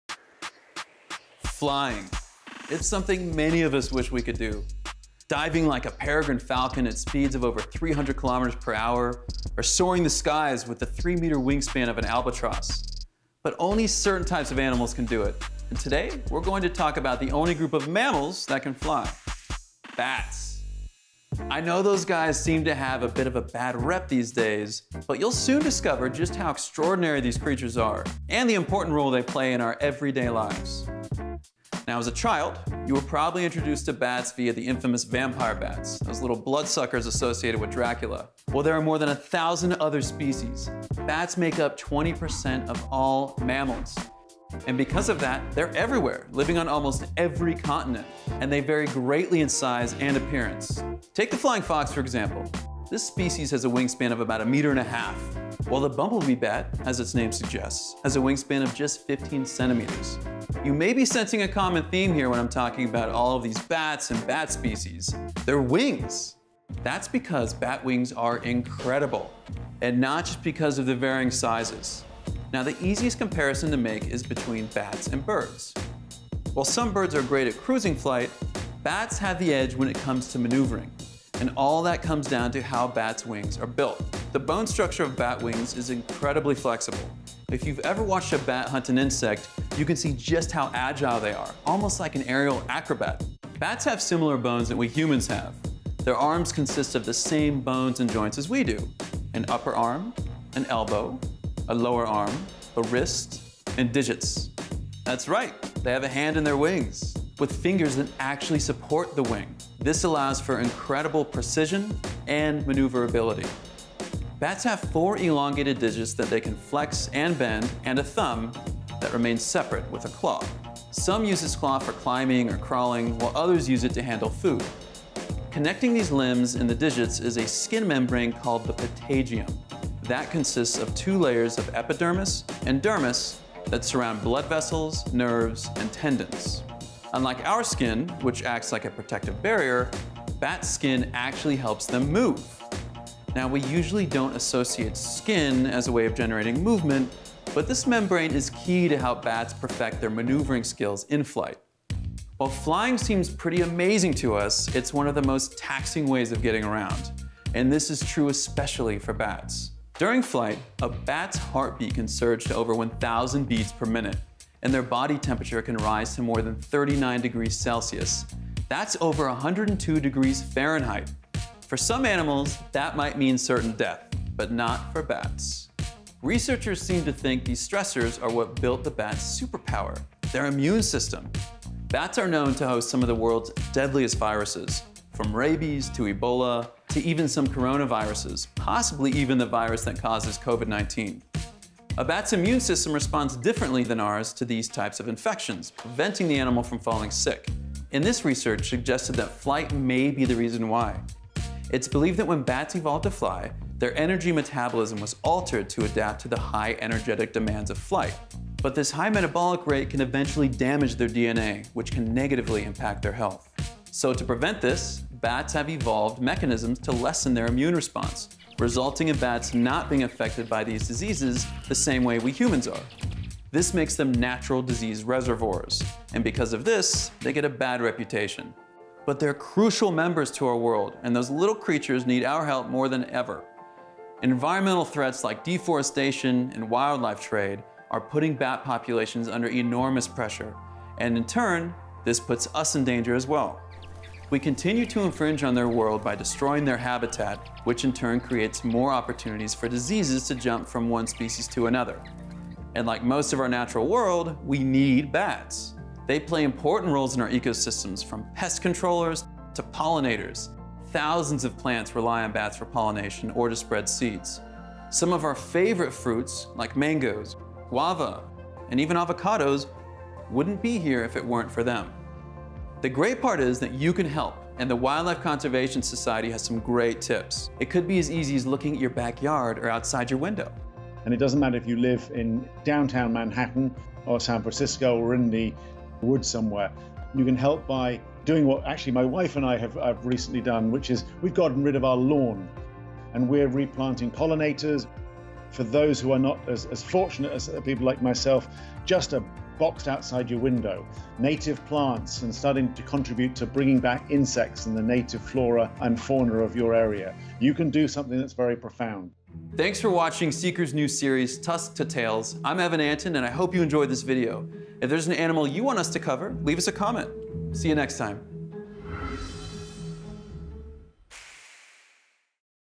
Tusk to Tails | The Only Mammals That Can Fly
Bats are the only mammals capable of true flight—and they’re way more fascinating than they get credit for. From acrobatic wings to an immune system built for survival, these creatures keep our ecosystems alive (and our fruit bowls full). 🦇🍃